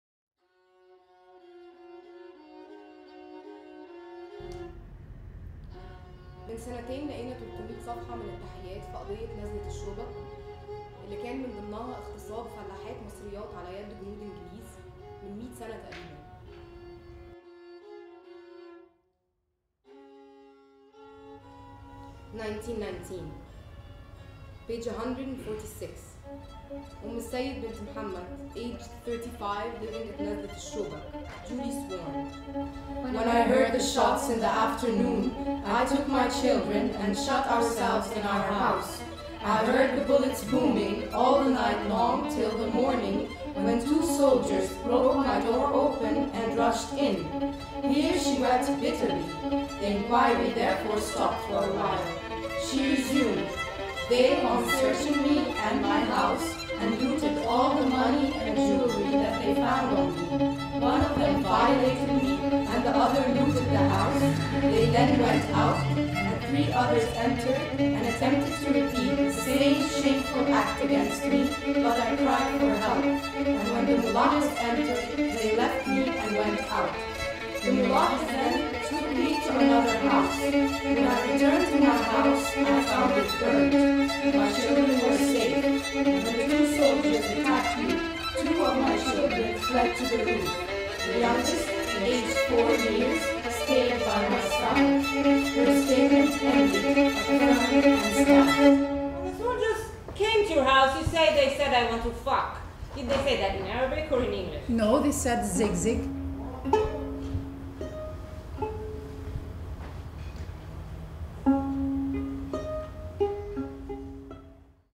Teaser en langue arabe sous-titré en anglais Laila Soliman / Zig Zig
Il y a presque cent ans, un petit village d’Égypte était pris pour cible par l’armée britannique. Lors du procès, fait rarissime, des femmes prennent la parole pour expliquer que les soldats les ont violées. Laila Soliman part de leurs témoignages pour évoquer ce moment historique et la persistance de la violence de genre qui le sous-tend.
Nouveau théâtre de Montreuil, centre dramatique national
12 au 21 octobre